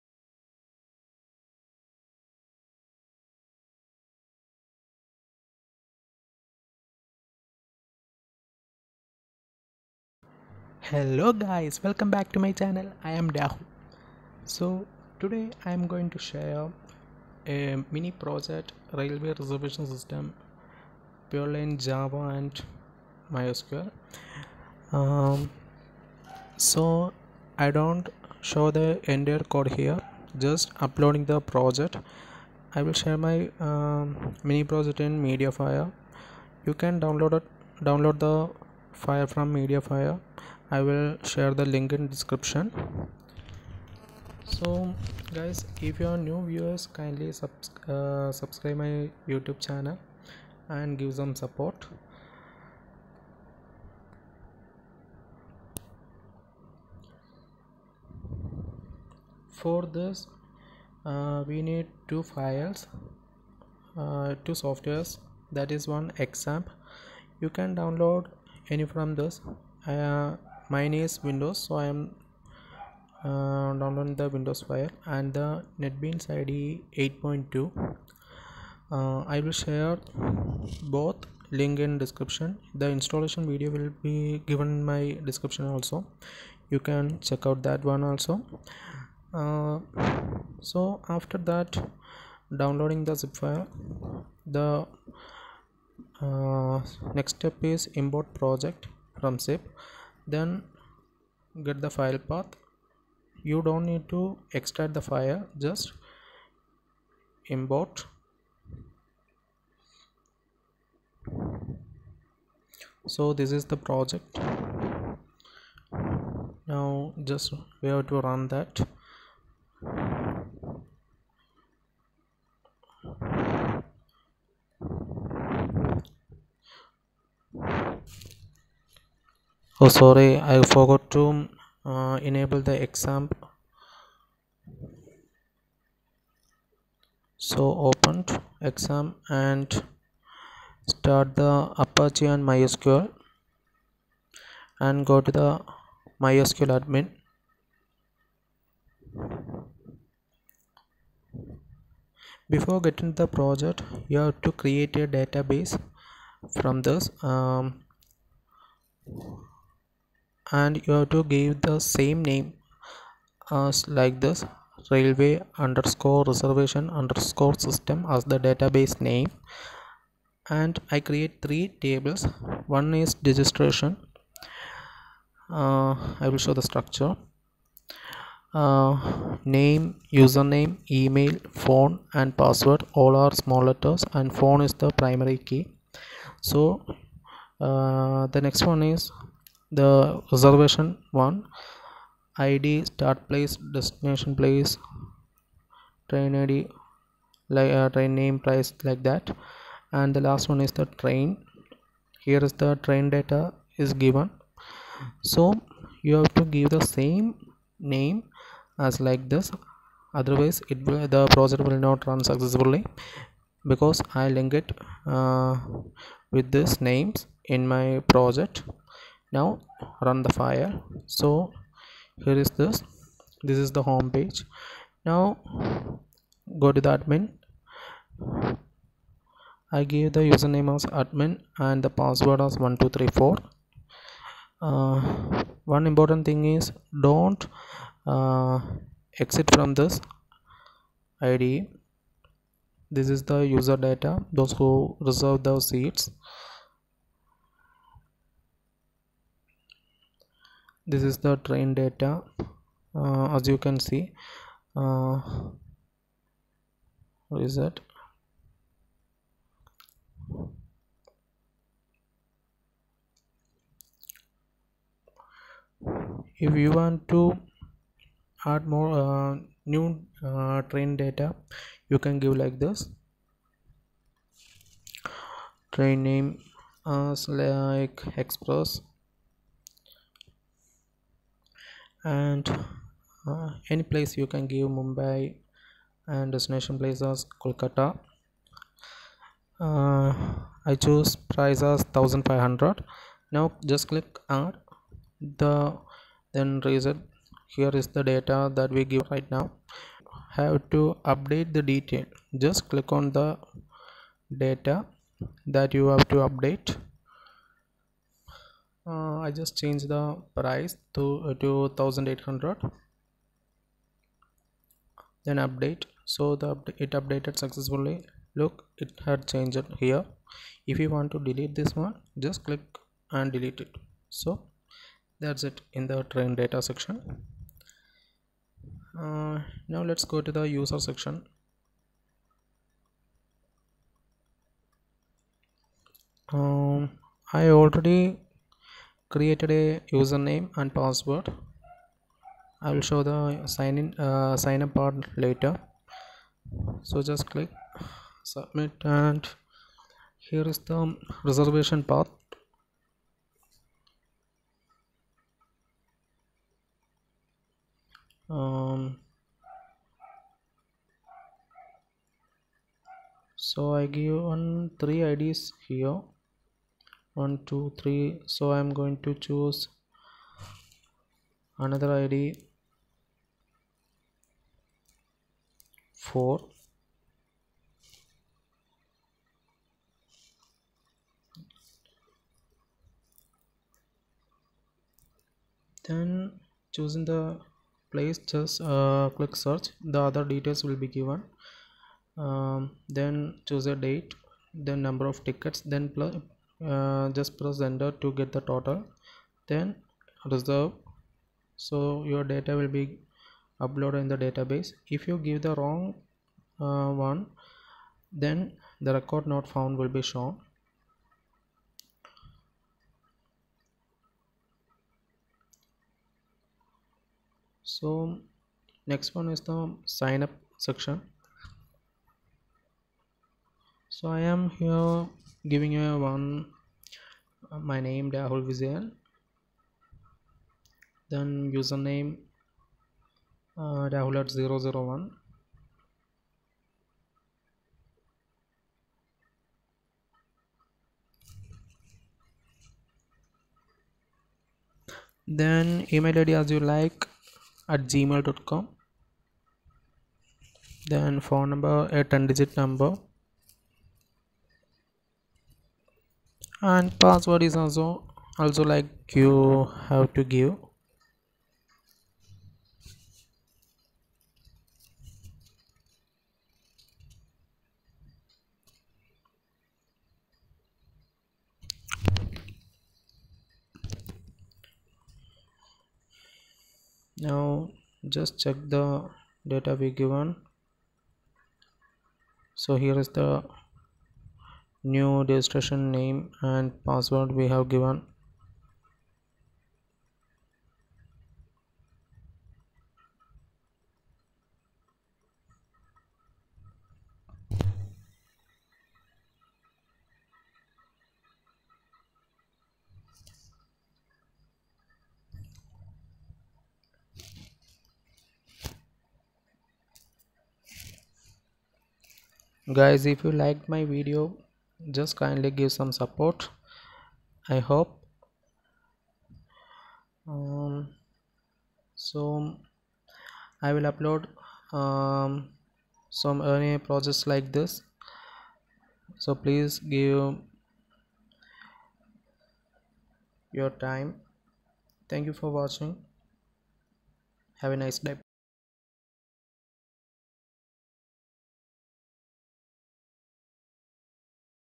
Railway Reservation System Using Java & MySQL - MINI PROJECT - With SourceCode @Rahul_Vijayan
Mini Project for Computer Science